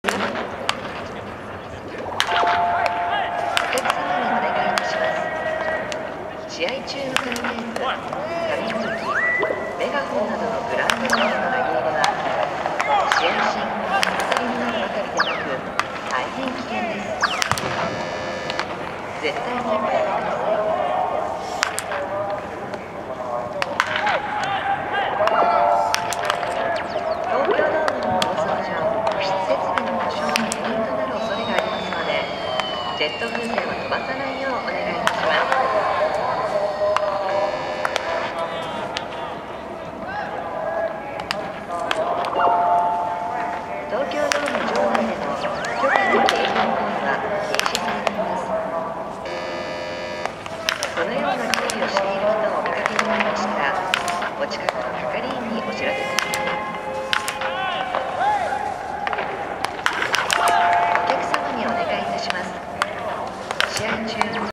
ヤクルト中村悠平のトスバッティング【2021年3月3日読売ジャイアンツVSヤクルトスワローズ】
2021年3月3日読売ジャイアンツ対ヤクルトスワローズの試合
再生リスト